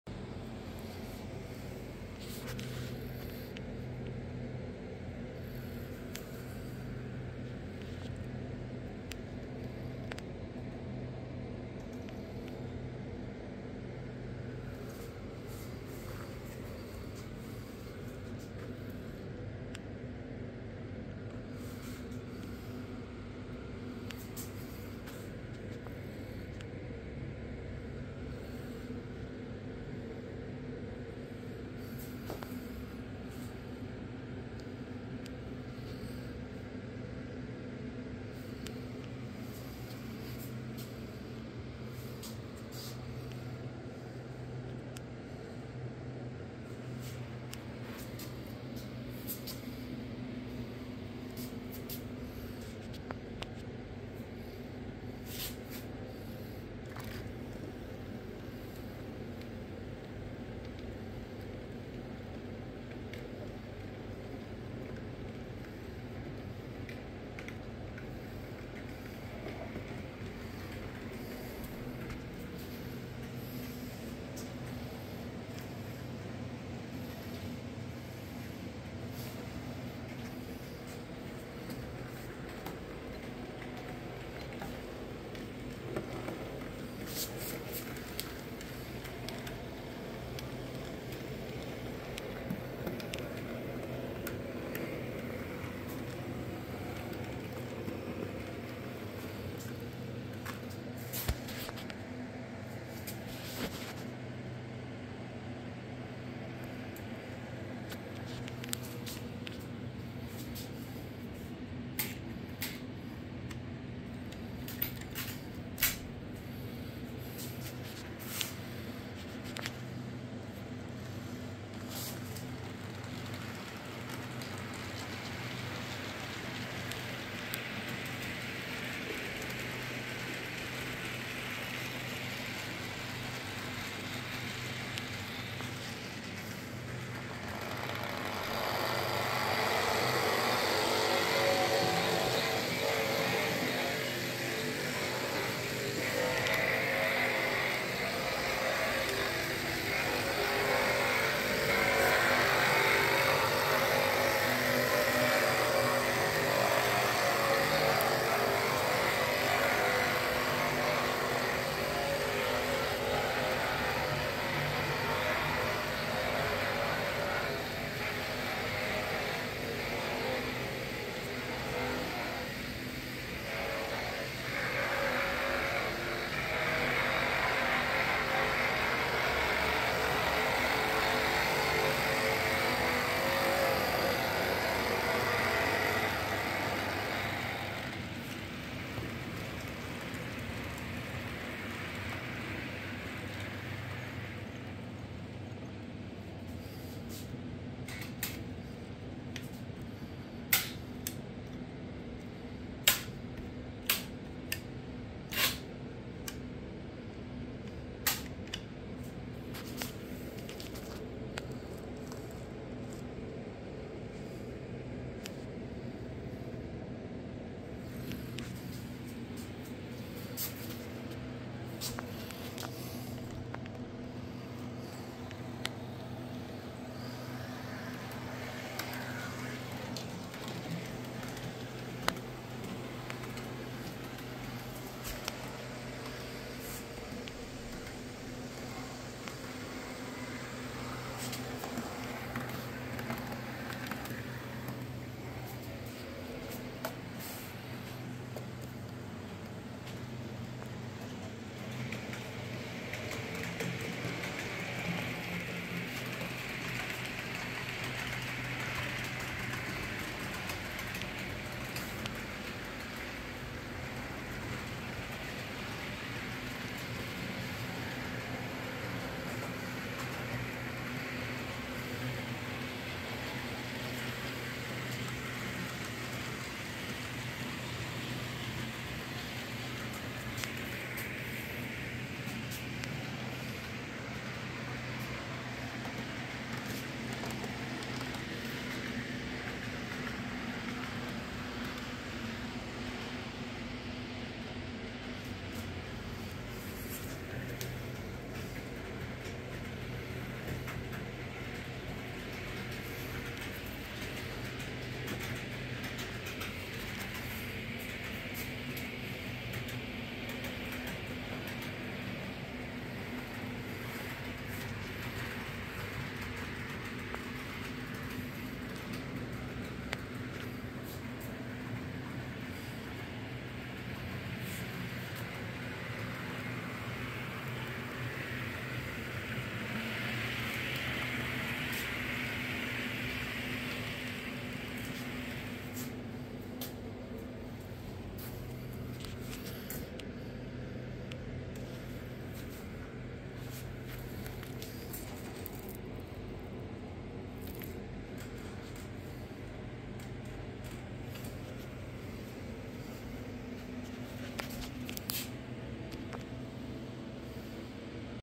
Klassische Kleinbahnmodelle in Betrieb
Die Firma Kleinbahn ist seit 2021 leider Geschichte, mir dem digitalen Fortschritt konnte man leider nicht mithalten. Ein Grund mehr einige Klassiker zu zeigen, für mich ist der Rote Pfeil der SBB natürlich ein "must have".